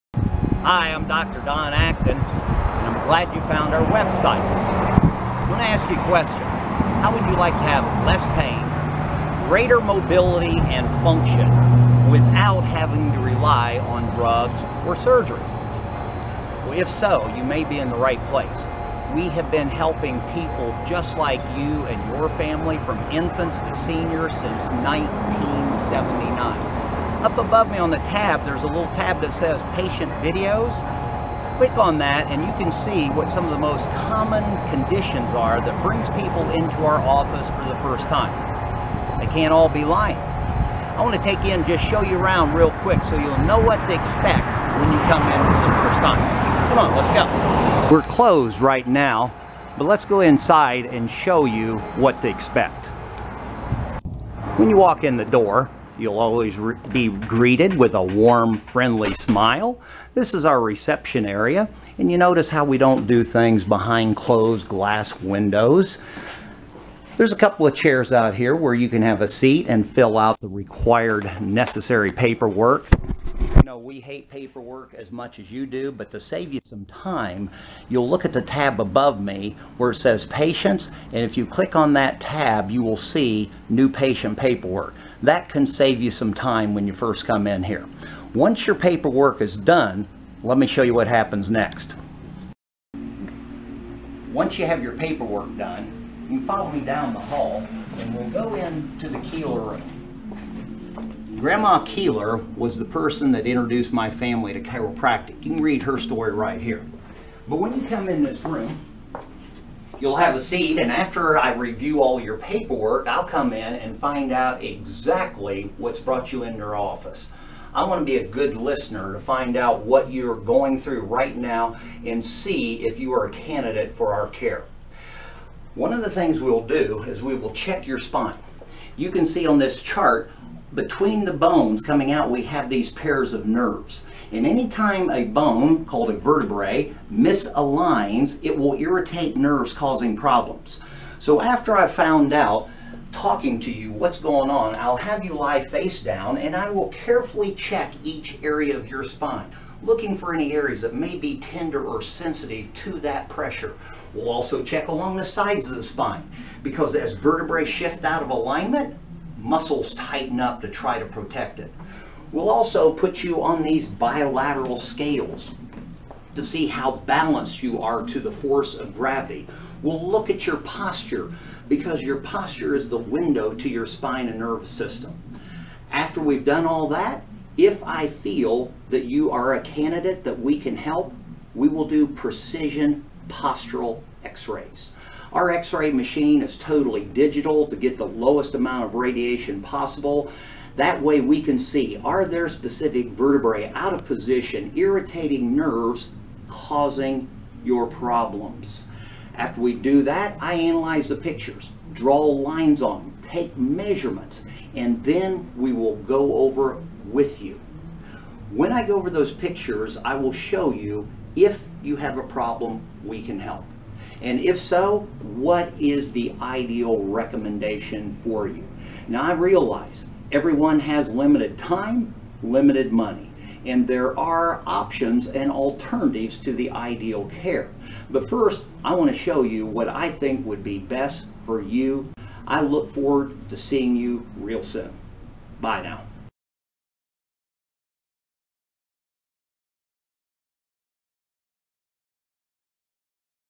Acton Family Chiropractic Office Tour | Asheville NC | Dr. Donald Acton
Dr. Donald Acton at Acton Family Chiropractic in Asheville NC provides an office tour.